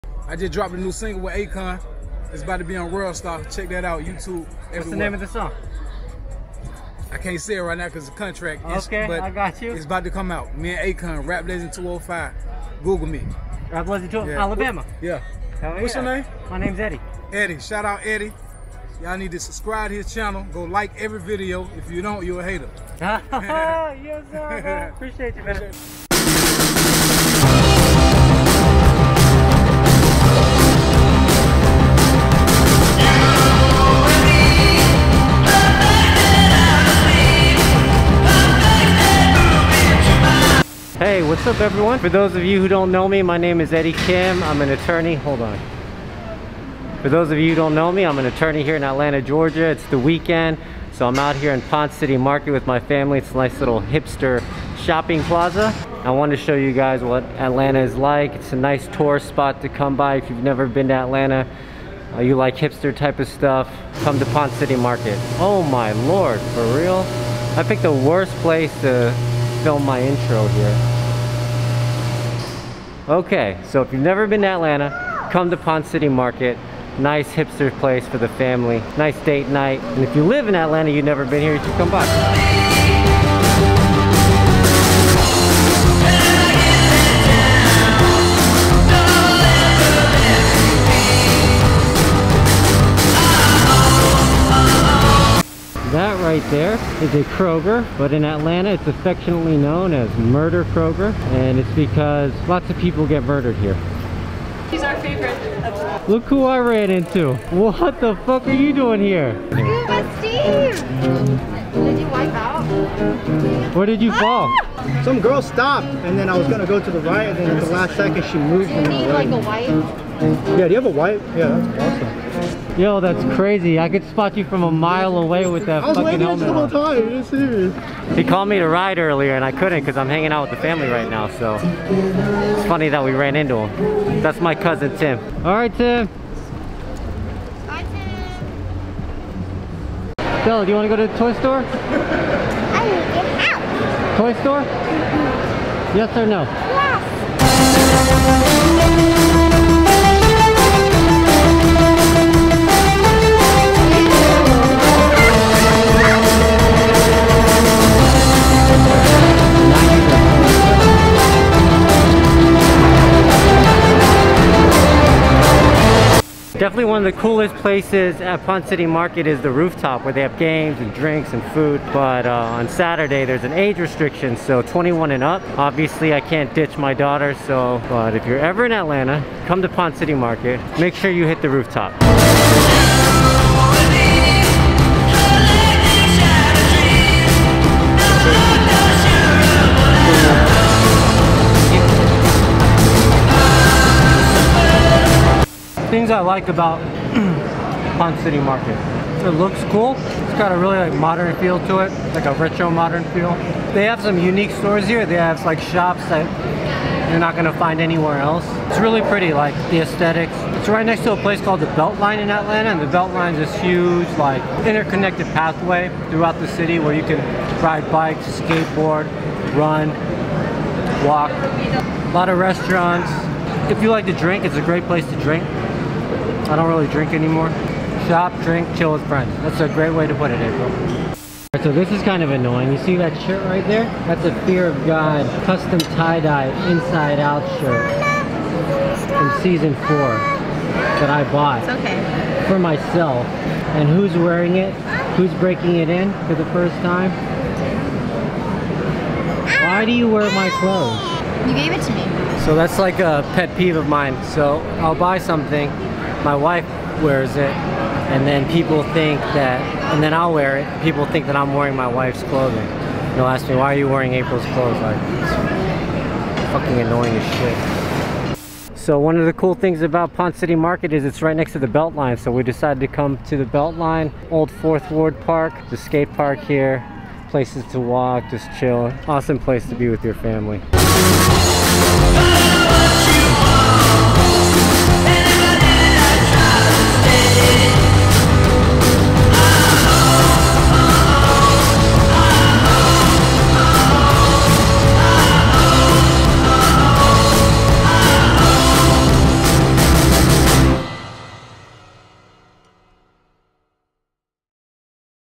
Welcome To Ponce City Market: A Hipster’s Paradise
If you’re looking for things to do in Atlanta, come check out Ponce City Marker in downtown Atlanta. PCM is located in an area known as Old Fourth Ward. O4W is a pocket of Atlanta known for its thriving hipster culture. Ponce City Market is the ultimate hipster shopping mall. You’ll find some of the typical malls stores, but it’s a great alternative shopping experience. The food court is one of a kind. You won’t find an Orange Julius or Dairy Queen at PCM. The rooftop also has carnival games and rides with an incredible view of the city. Fun for all ages. Check out the vlog to see what kind of fun we get into at Ponce City Market

As you guys know, I’m an attorney in Atlanta.